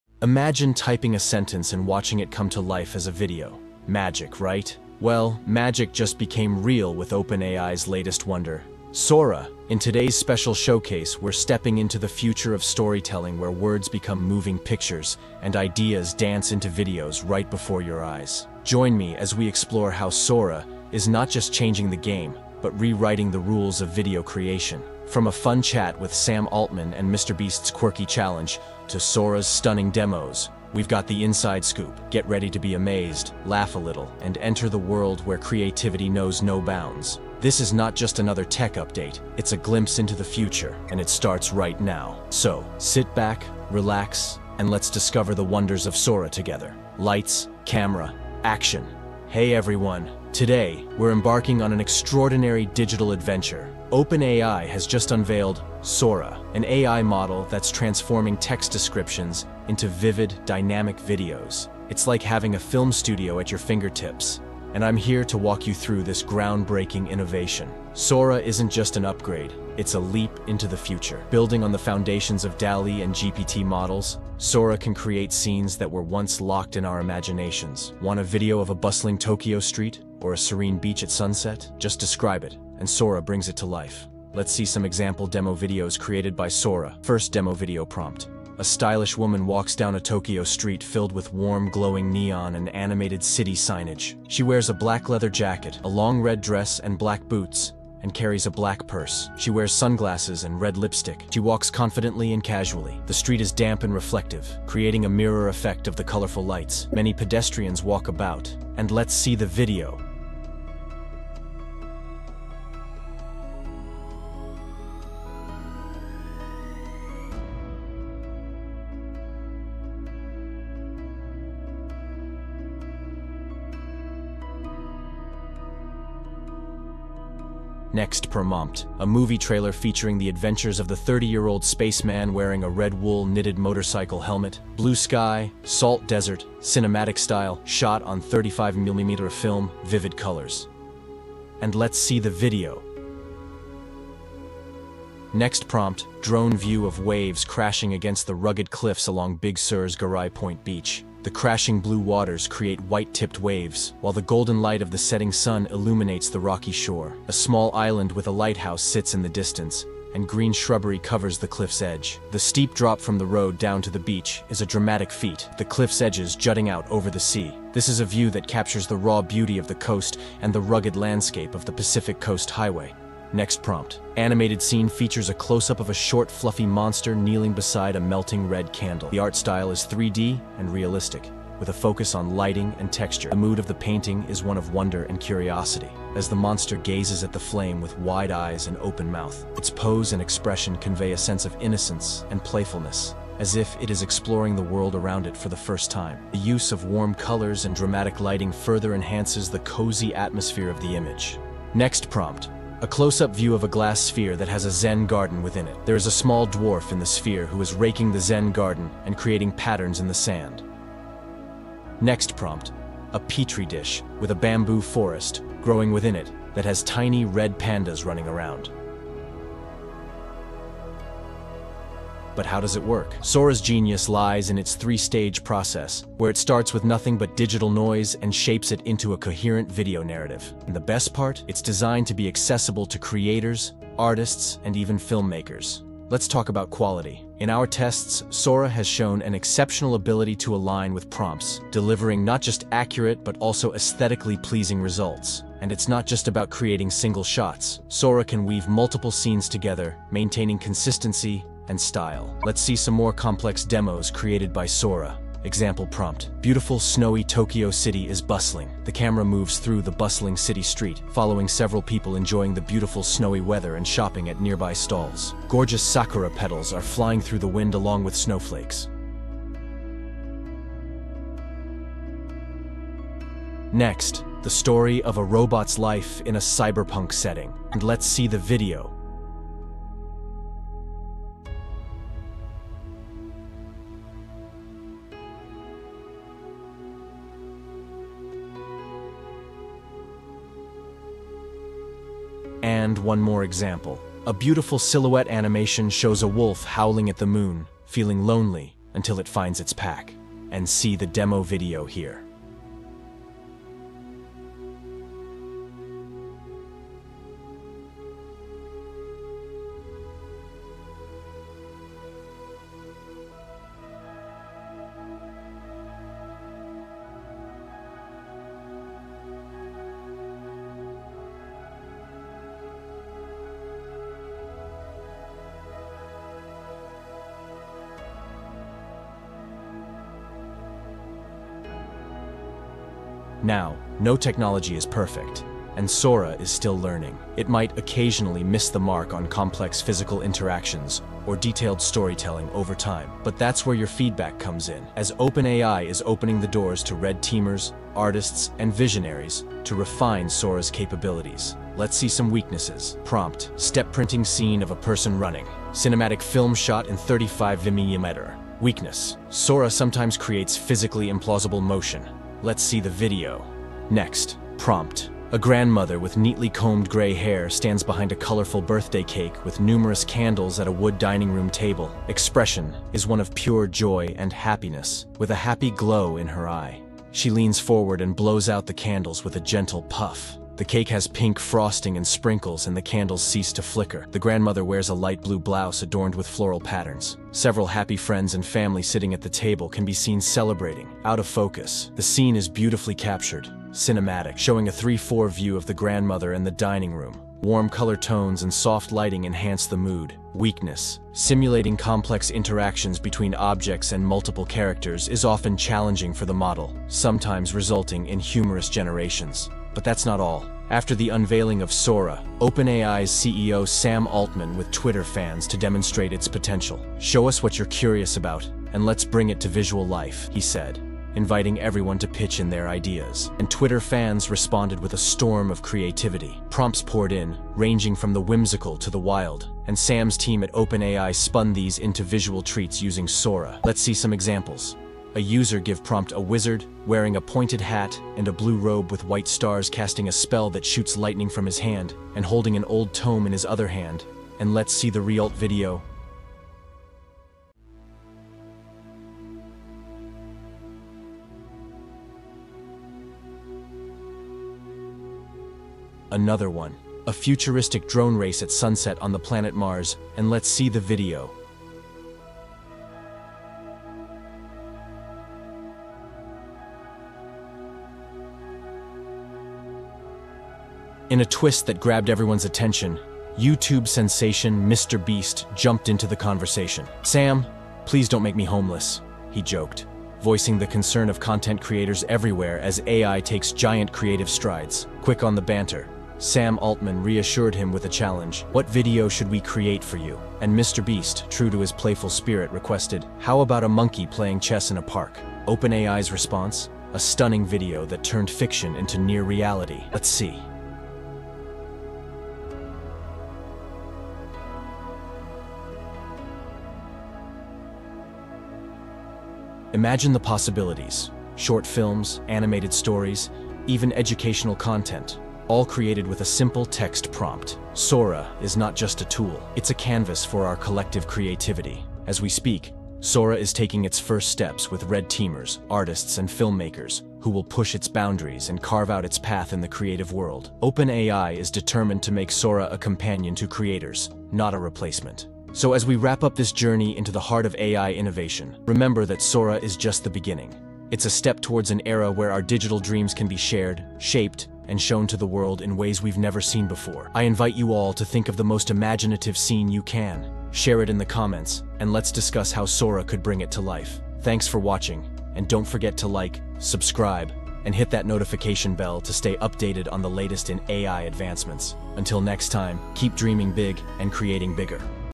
OpenAI's Sora: Transforming Video Production with Text-to-Video AI Model
"🌟 Dive into the groundbreaking world of OpenAI's latest innovation, Sora - a cutting-edge AI model that's redefining the landscape of video production. From converting simple text prompts into elaborate video scenes to discussions between Sam Altman and MrBeast on the future of content creation, this video covers it all. Sora represents a significant leap towards merging creativity with AI, offering creators, artists, and filmmakers unprecedented tools to bring their imaginations to life. Explore the capabilities, demonstrations, and potential of Sora, and how it stands to change the industry forever.

Drop your thoughts and questions in the comments below!

💬 Share your thoughts or suggest projects for our next review in the comments!"